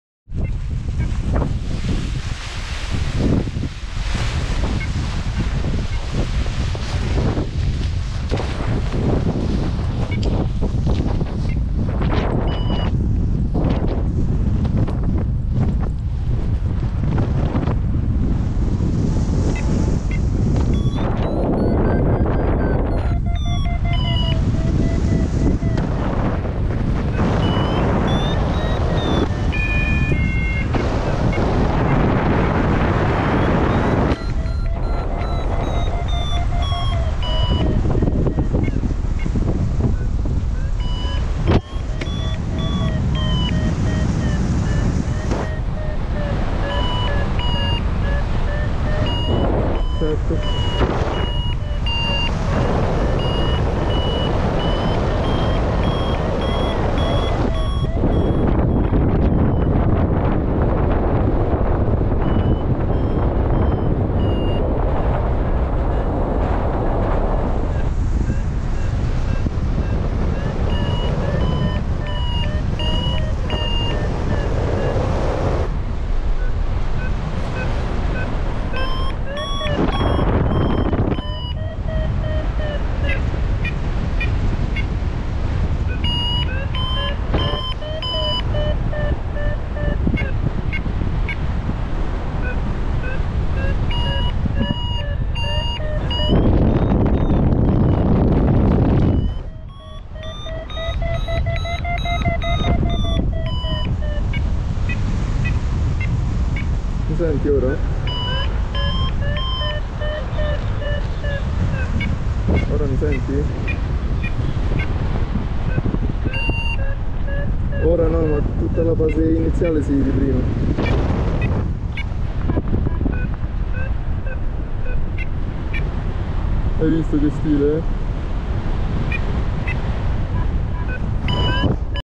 Cobra Launch in Strong Wind (27 km/h) with Ozone Photon
Before getting ready the wind was really calm and it was 3 pm already. Usually at that time in Tivoli there's the sea breeze coming inland so it's usually strong.
I went to the takeoff area with another pilot and there was a cumulus that shaded the whole area.
A lot of beautiful cu's were forming all around and the thermic cycle was perfect.
The sun came back and we were about to launch but a strong wind kicked in or so we thought.
Actually that was a dust devil that was strong enough to lift my mate up and drag him for a few meters (I'll upload the short!)
After that a really strong wind really kicked in, an average of 27 km/h, spiking at 30 or so.

Lately I've been doing the same flights with the same conditions so I decided that it was time to do something different and train in stronger winds.
I waited patiently for a safe window to launch and I took off.